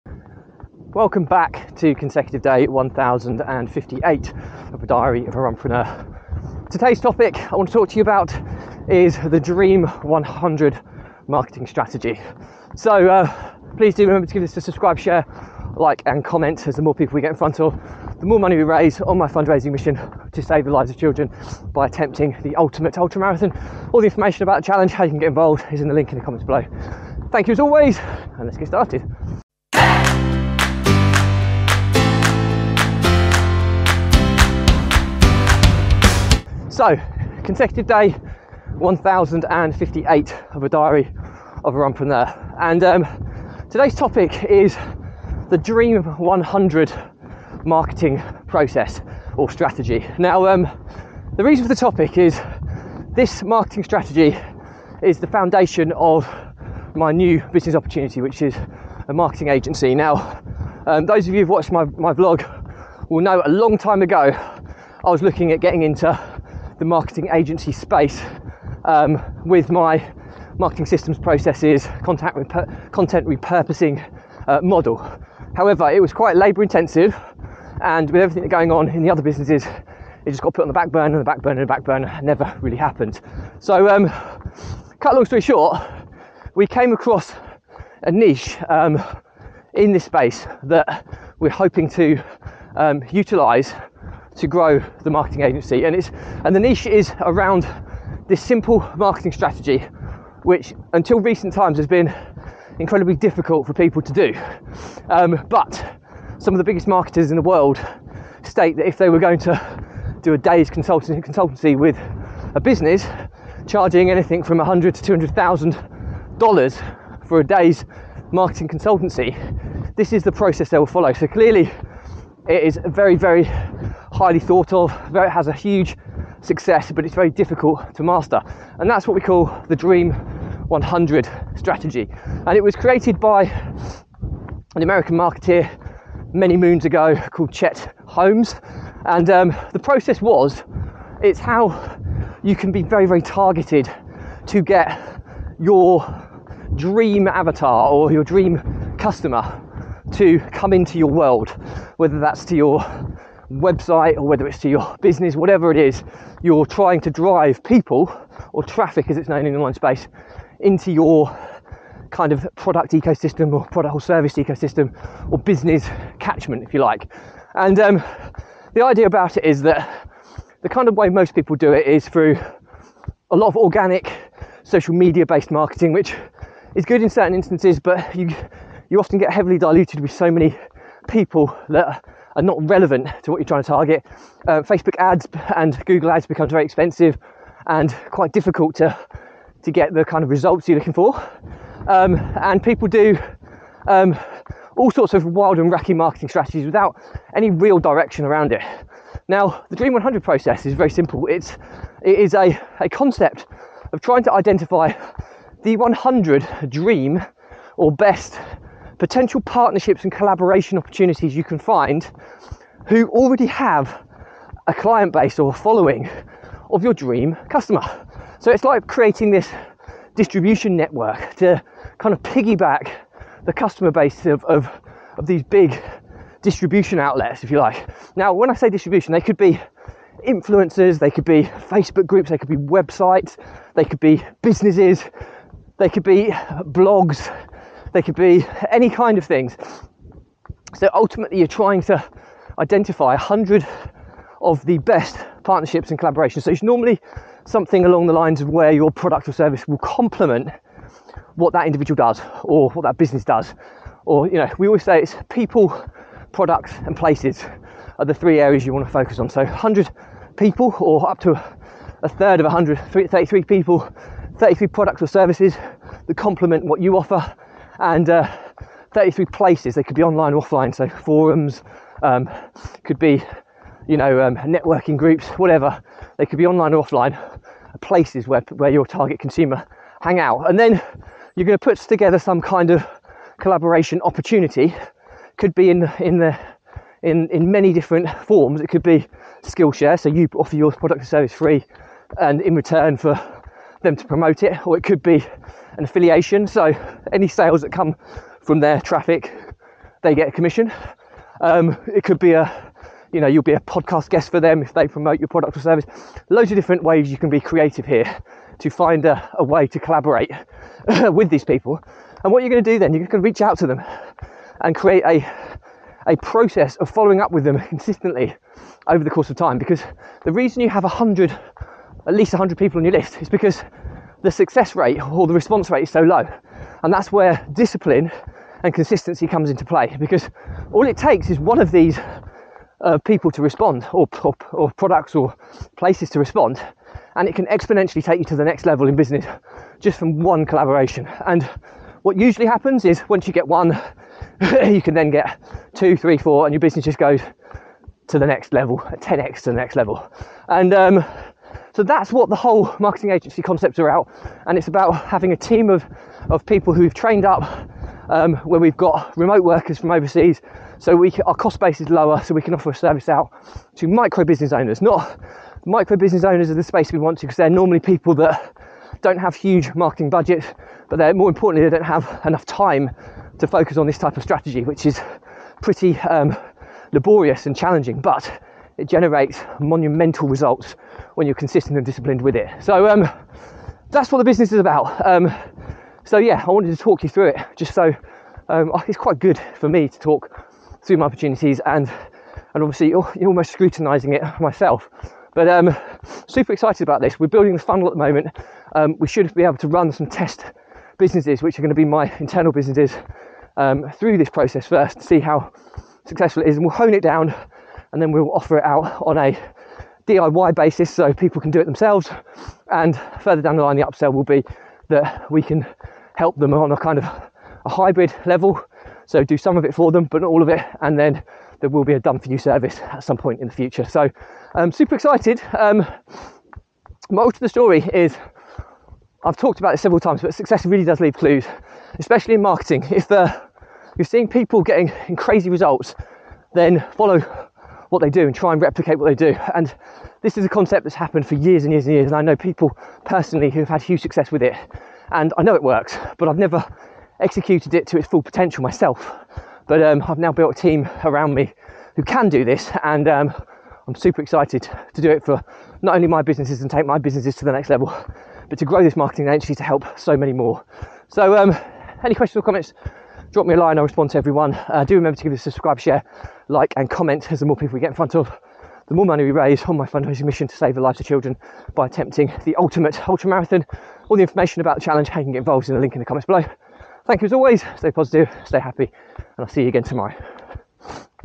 Unlocking the Power of the Dream 100 Marketing Strategy | Diary Of The Runpreneur 1058/4292
Welcome back to my journey on consecutive day 1058! Today, I'm diving into the Dream 100 marketing strategy and how it forms the cornerstone of my new marketing agency venture. Learn about this powerful approach developed by Chet Holmes and how it can elevate your business to new heights through targeted collaborations and partnerships.
I'll explain the simplicity, yet effectiveness, of the Dream 100 process and reveal how my team and I are implementing it to drive monumental results.
Check the link in the comments below for more info on how you can get involved.
Stay positive, stay happy, and I'll see you again tomorrow!